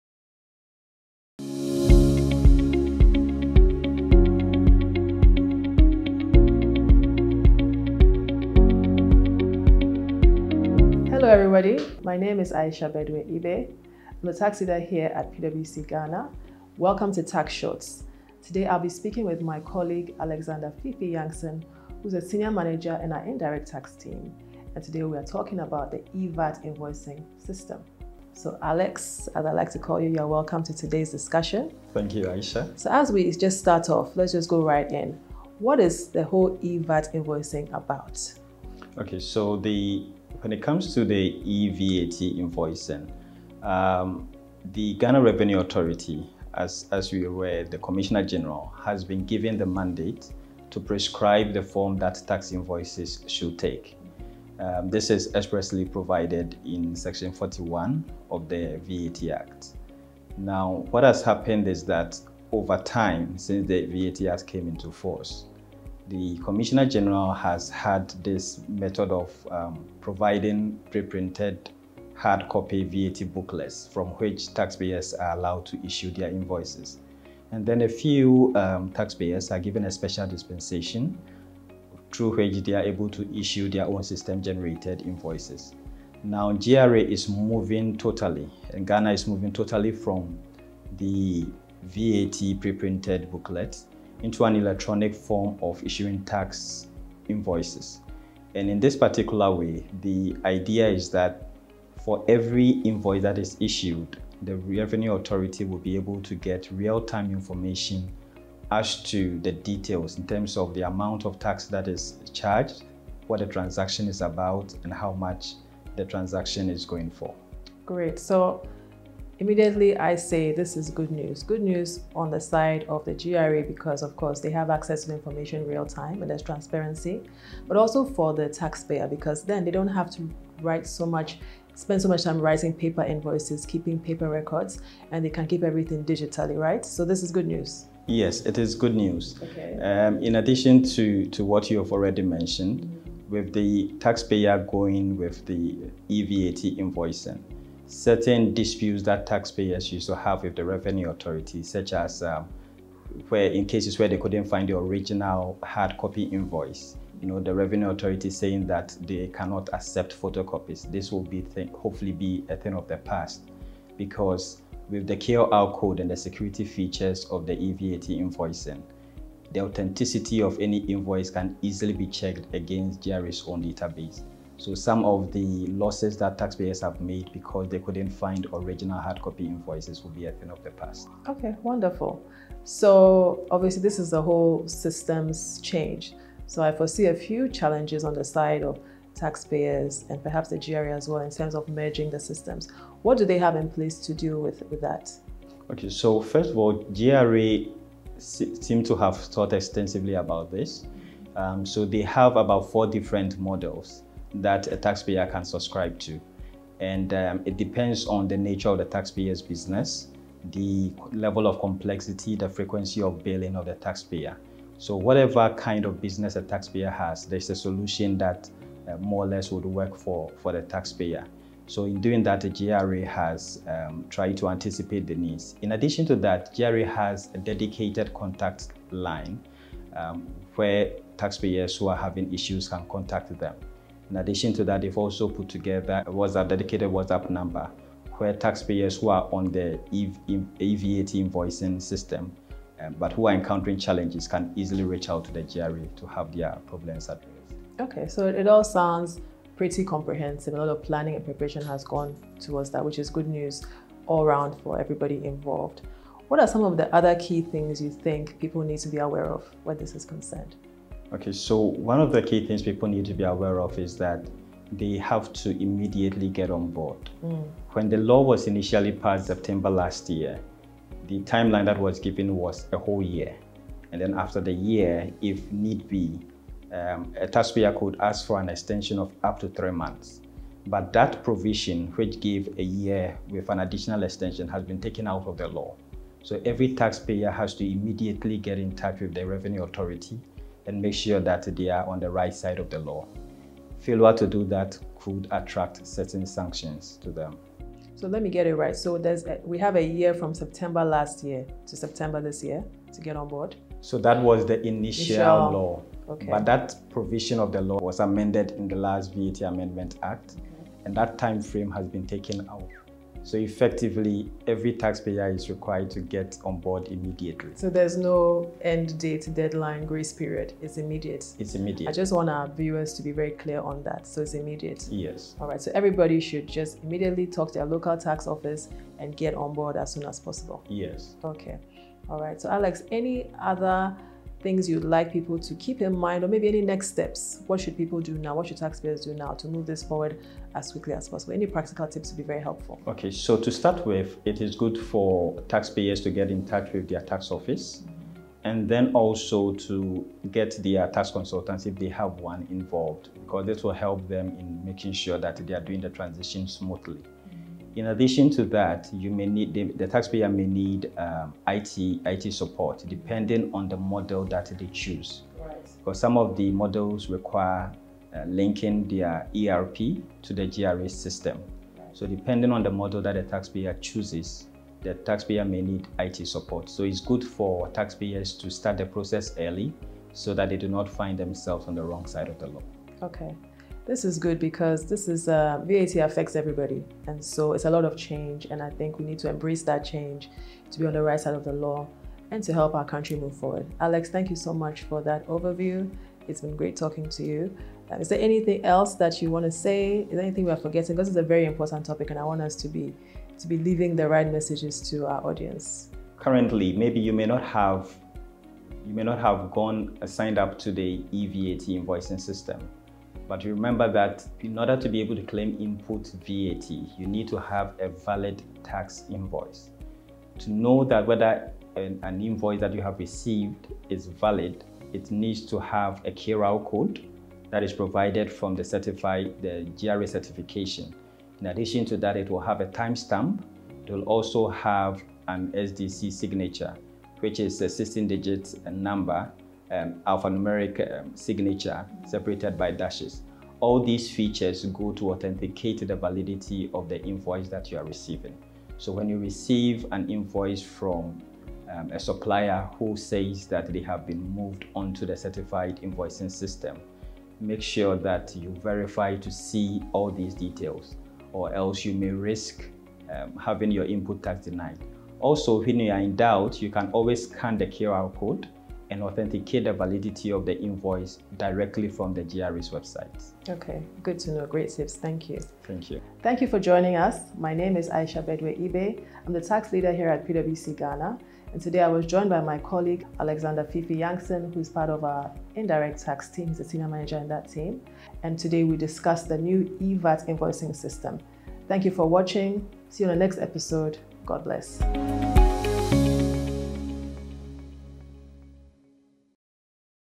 PwC Ghana Tax shorts: E-VAT invoicing system
PwC's Ayesha Bedwei Ibe discusses the introduction of E-VAT invoicing system in Ghana with PwC's Alexander Yankson.